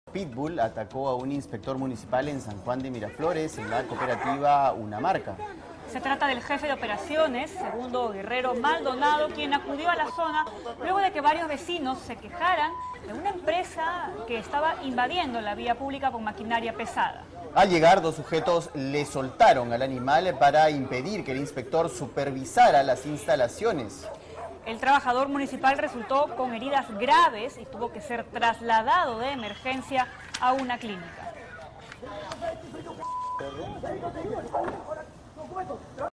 Inspector municipal resulta herido tras ser atacado por perro pitbull (2/2)
El trabajador se recupera en la clínica Santa María de Sur en San Juan de Miraflores.